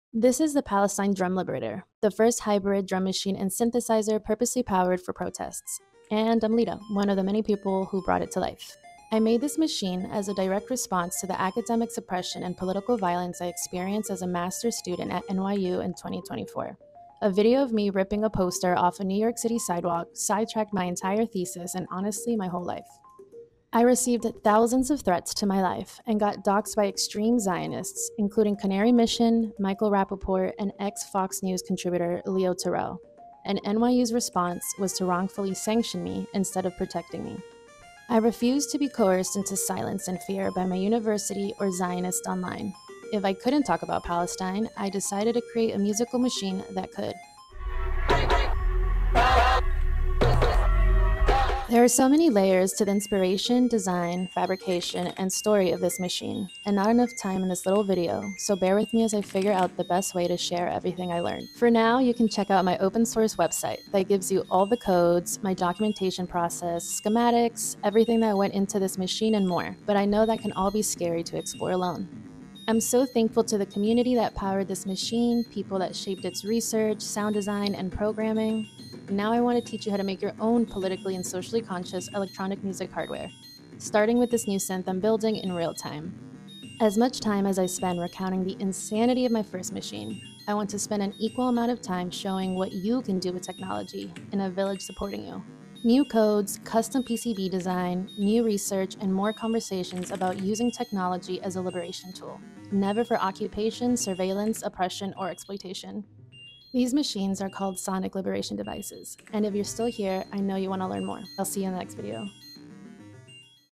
Protest Powered Drum Machine and Synth ✩ synthesizer tech music stem arduino diy hardware ✩
This is The Palestine Drum Liberator -- the first hybrid drummachine and synthesizer purposely powered for protests.

If you ever wanted to learn how to make diy homemade hardware, welcome to this space.

A new era in musictechnology is loading. Hope you join us.